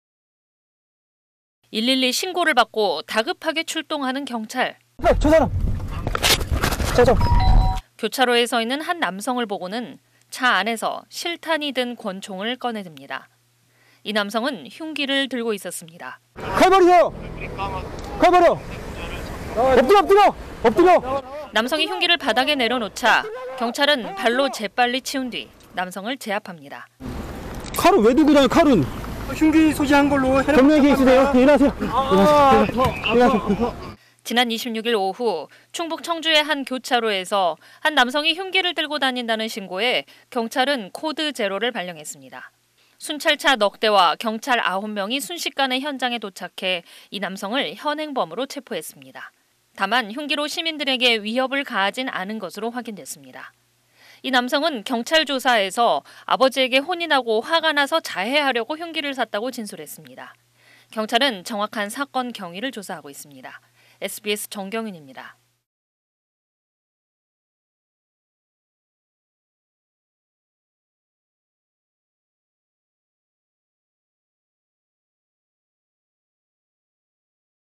"아버지에게 혼나서…" 술 취해 흉기 들고 배회한 40대 남성 검거 (자막뉴스) / SBS
112 신고를 받고 다급하게 출동한 경찰,

[경찰 : 저 사람, 저 사람!]

교차로에 서 있는 한 남성을 보고는 차 안에서 실탄이 든 권총을 꺼내 듭니다.

이 남성은 흉기를 들고 있었습니다.

[경찰 : 칼 버려! 칼 버려! 엎드려! 엎드려!]

남성이 흉기를 바닥에 내려놓자 경찰은 발로 재빨리 치운 뒤 남성을 제압합니다.

[경찰 : 칼을 왜 들고 다녀, 칼을.. 흉기 소지한 걸로 현행범 체포하는 거예요.]

[피의자 : 아아아 아퍼.]

지난 26일 오후, 충북 청주의 한 교차로에서 한 남성이 흉기를 들고 다닌다는 신고에, 경찰은 '코드 제로'를 발령했습니다.

순찰차 4대와 경찰 9명이 순식간에 현장에 도착해 이 남성을 현행범으로 체포한 겁니다.

다만 흉기로 시민들에게 위협을 가하진 않은 것으로 확인됐습니다.

이 남성은 경찰 조사에서 아버지에게 혼나고 화가 나서 자해하려고 흉기를 샀다고 진술했습니다.

경찰은 정확한 사건 경위를 조사하고 있습니다.

(화면제공 : 충북경찰청 / 영상편집 : 서지윤 / 제작 : 디지털뉴스편집부)